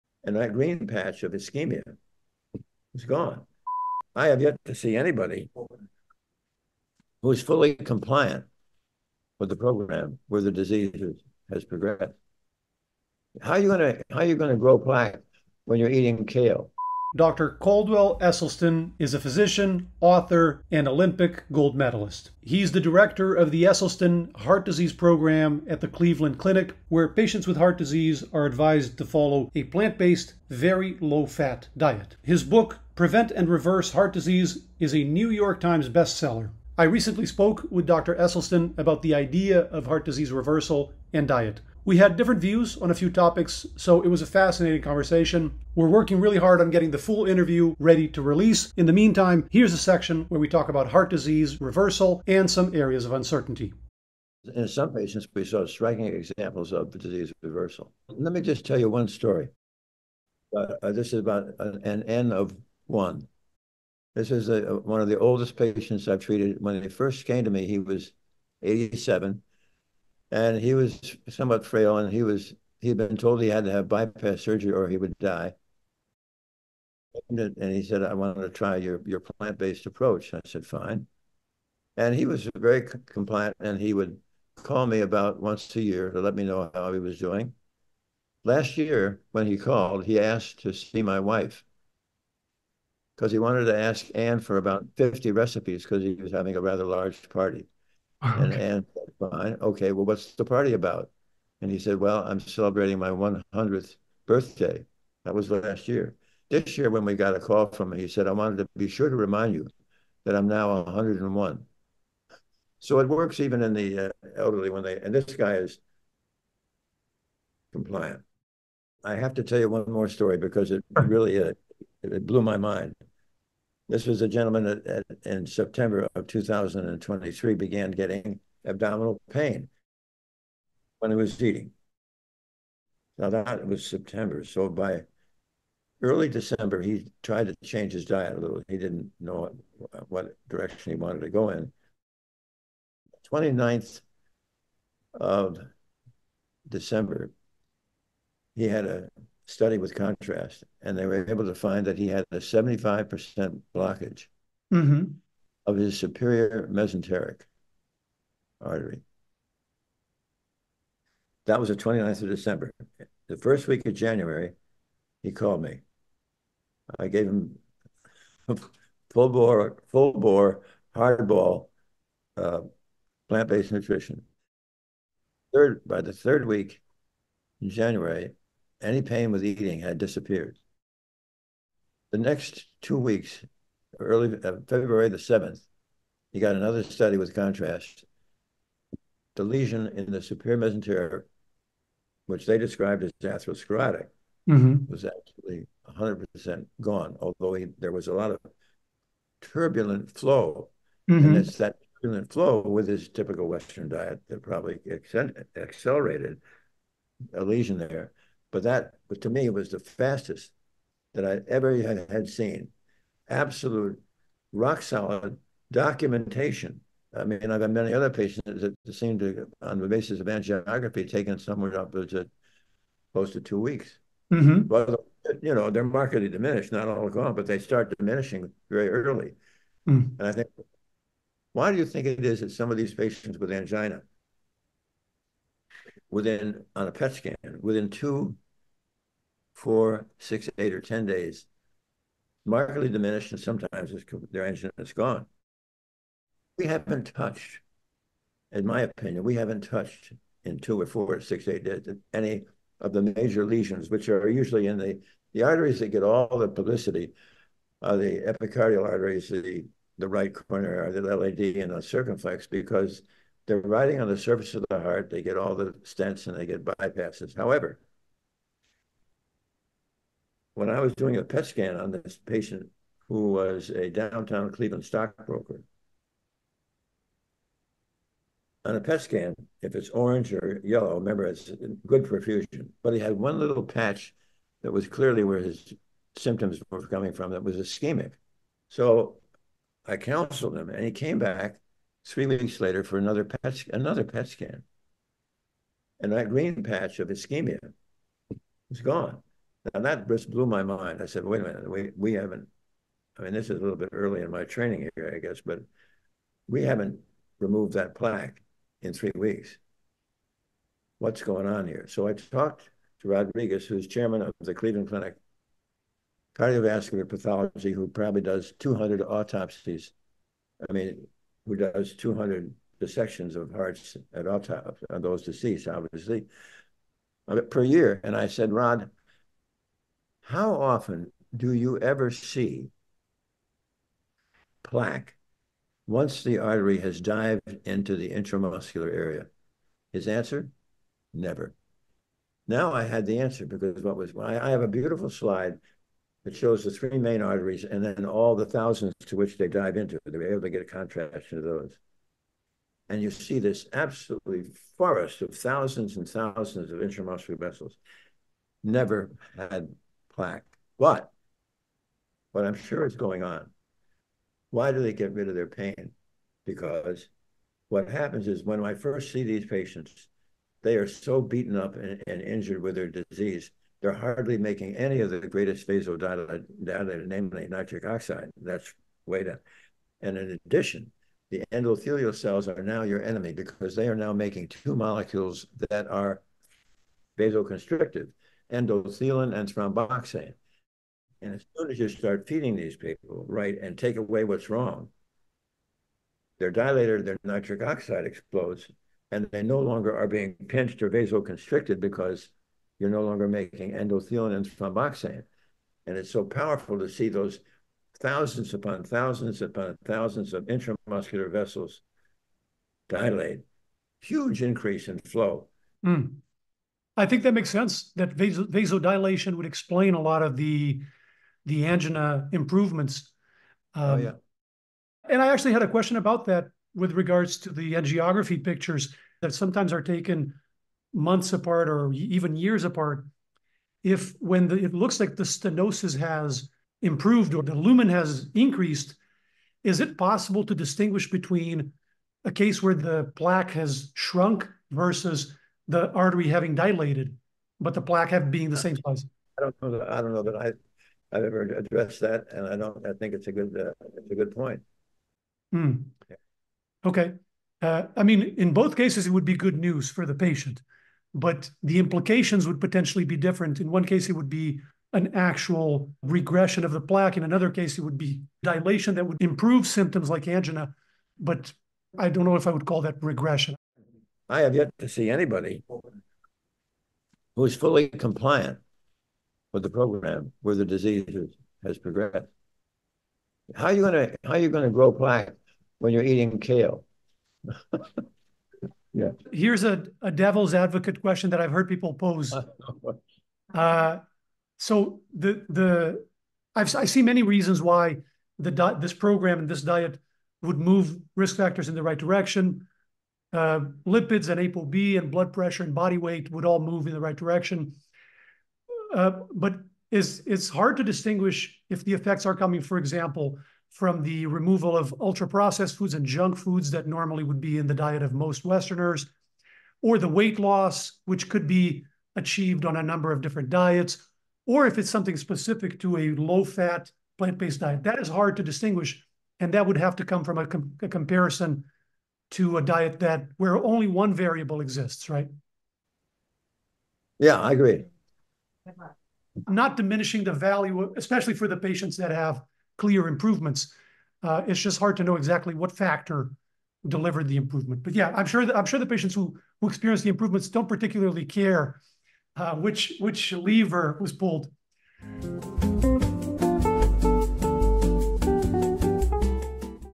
Reversing Heart Disease with a Vegan Low fat Diet? | Dr. Caldwell Esselstyn MD
I spoke with Dr. Caldwell Esselstyn from the Cleveland Clinic about Reversing Heart Disease and vegan low fat diets.

Do imaging studies show actual heart disease reversal?

Is a low fat vegan diet required for heart disease patients?

0:00 Dr. Esselstyn
1:16 Centenarian patient
2:09 Abdominal pain patient
3:52 Rapid results
7:20 Regression vs dilation
8:10 A devil´s advocate